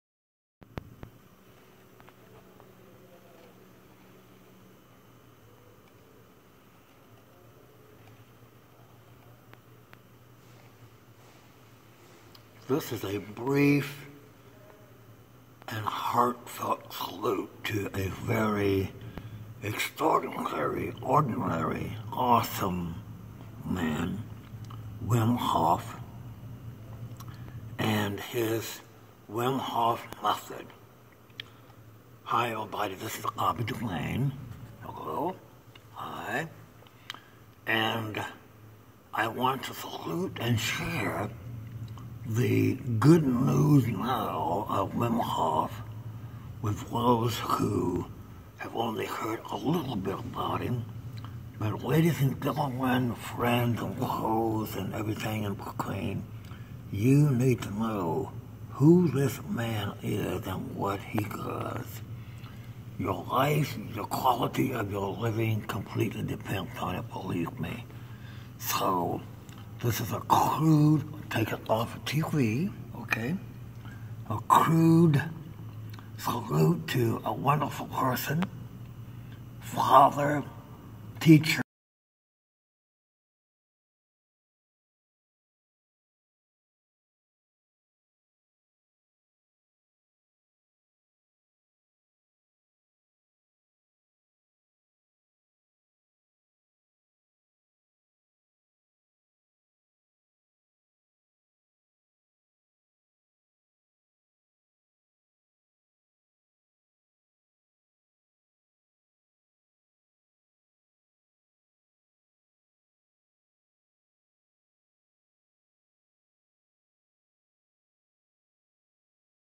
AN APPRECIATION AND SALUTE TO WIM HOF
VLOGR20AOUT2020
this is a brief appreciation to this extraordinary man for his discovery of what is an easy and powerful means of self healing.

i am including his inspiration in all my research and clinical/studio work.